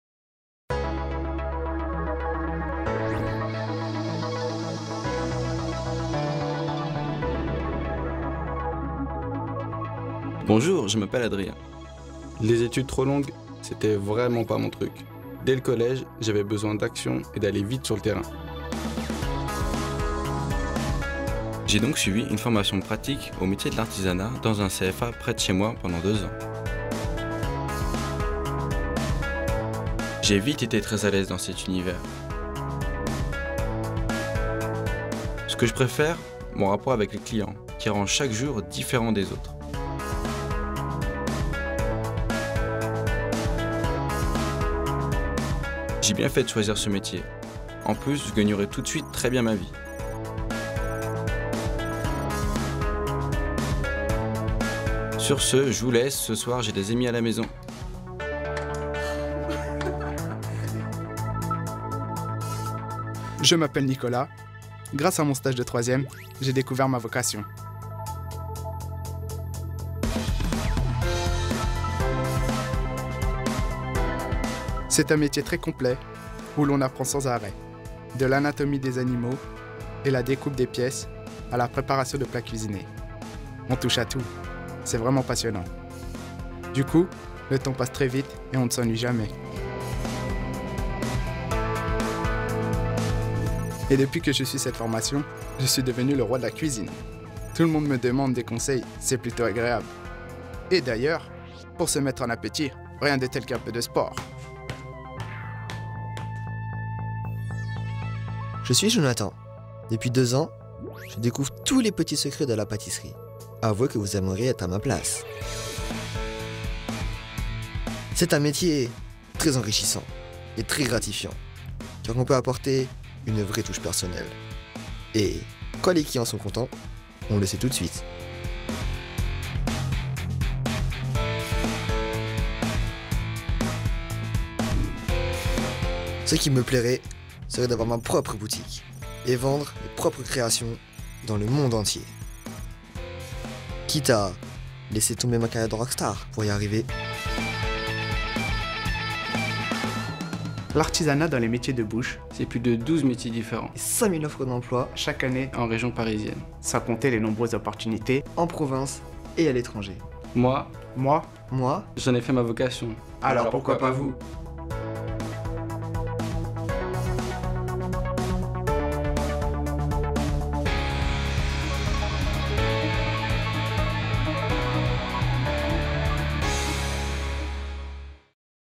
L'apprentissage des métiers de bouche
. L'artisanat dans les métiers de bouche, c'est plus de 12 métiers différents, 5000 offres d'emploi chaque année en région parisienne, sans compter les nombreuses opportunités en province et à l'étranger.
Adrien, Nicolas et Jonathan en ont fait leur vocation.
Jeunes du Val-de-Marne, venez découvrir les différents métiers de l'artisanat